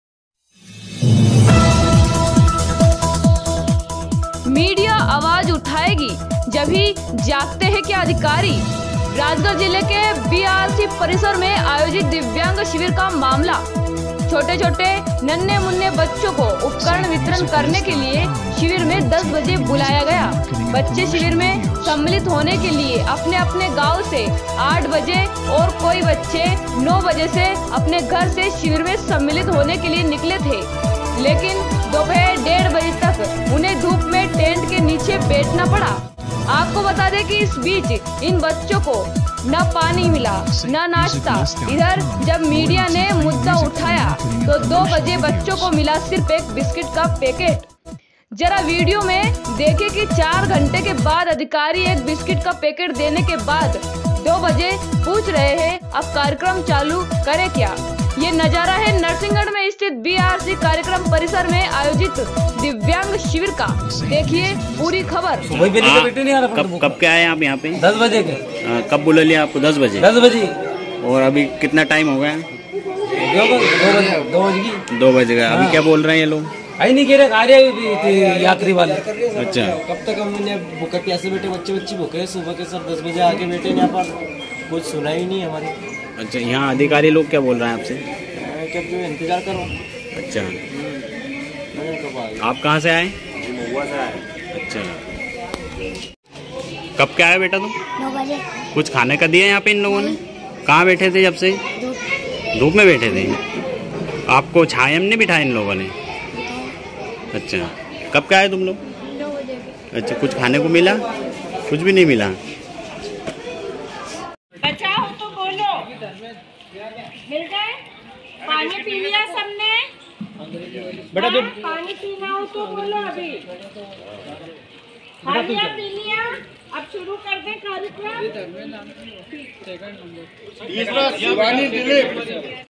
नरसिंहगढ़ में आयोजित दिव्यांग शिविर का मामला,मीडिया आवाज उठाएगी, जब ही जागते हैं क्या अधिकारी ?
*मीडिया आवाज उठाएगी, जब ही जागते हैं क्या अधिकारी*
👉नरसिंहगढ़ BRC परिसर में आयोजित दिव्यांग शिविर का मामला